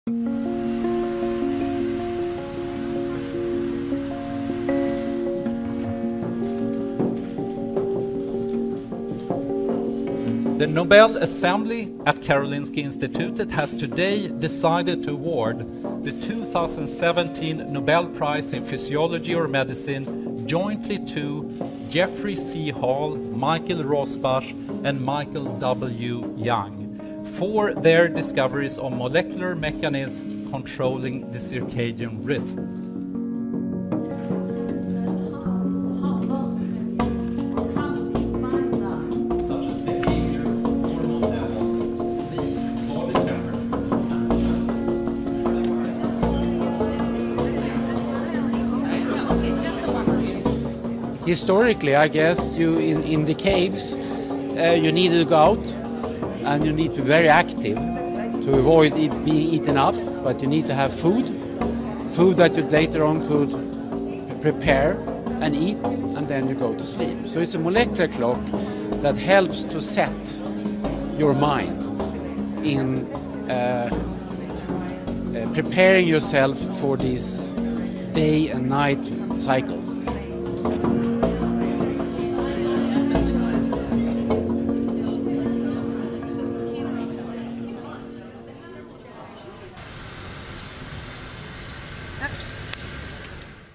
Nobel de Medicina 2017 para los descubridores del 'reloj interno' | Ciencia
Los científicos Jeffrey C. Hall, Michael Rosbash y Michael W. Young han ganado hoy el premio Nobel de Medicina de 2017 "por sus descubrimientos de los mecanismos moleculares que controlan el ritmo circadiano"
Más